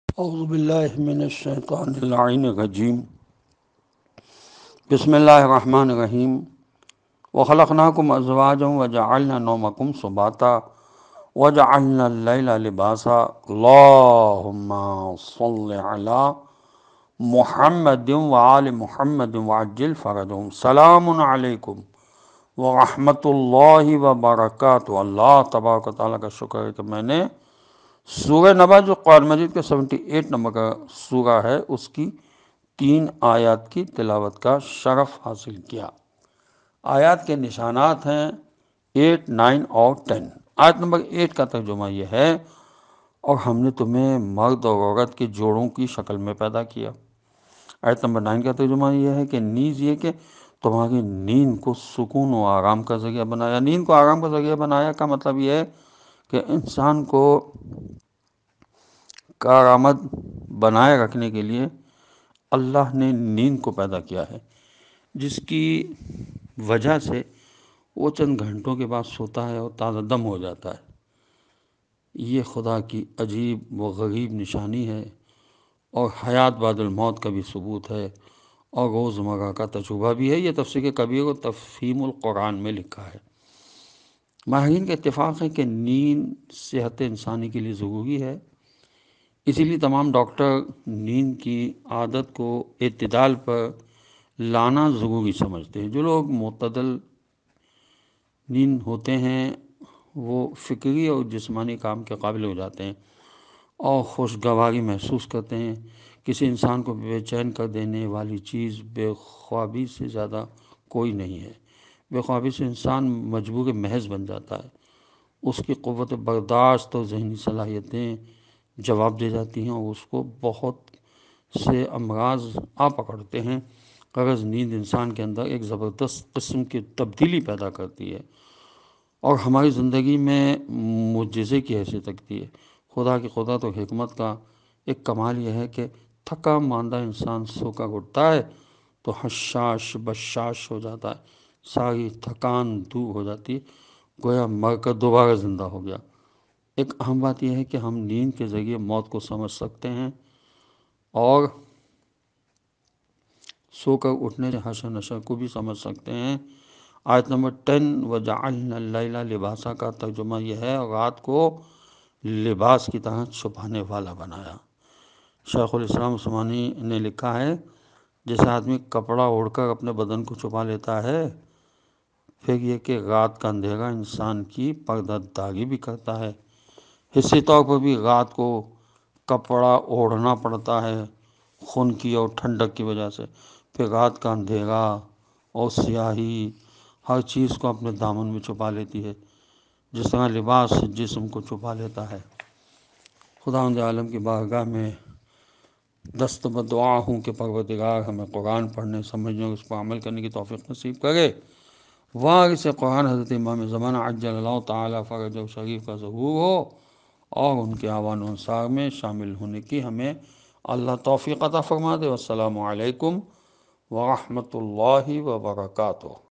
Surah Naba Verses 8-10 | Urdu Translation Tafseer By Maulana Ali Ahmed | Holy Quran Motivation
Surah Naba Verses 8-10